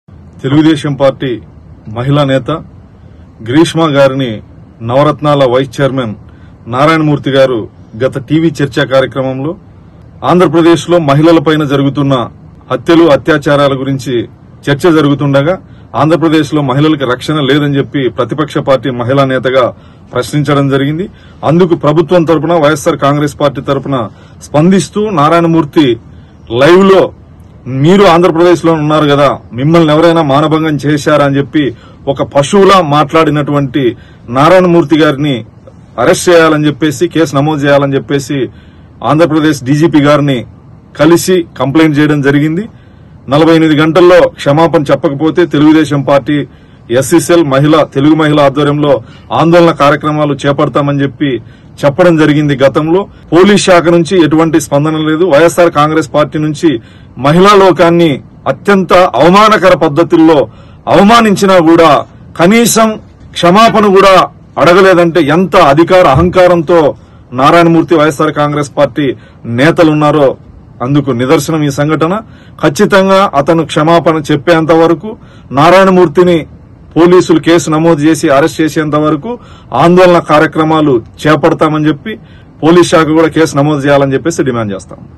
18 April 2022
ఒక టీవీ చర్చా కార్యక్రమంలో తెలుగుదేశం దళిత మహిళా నేత కావలి గ్రీష్మ పట్ల అనుచిత వ్యాఖ్యలు చేసిన అనాగరిక వైసీపీ నేత పై ప్రభుత్వం కానీ, పోలీసులు కానీ ఇంతవరకు ఎలాంటి చర్యలు తీసుకోలేదని... అయితే తాము మాత్రం ఆయన్ను వదిలేది లేదని అంటున్నారు తెలుగుదేశం ఎస్సీ సెల్‌ అధ్యక్షుడు ఎమ్మెస్‌ రాజు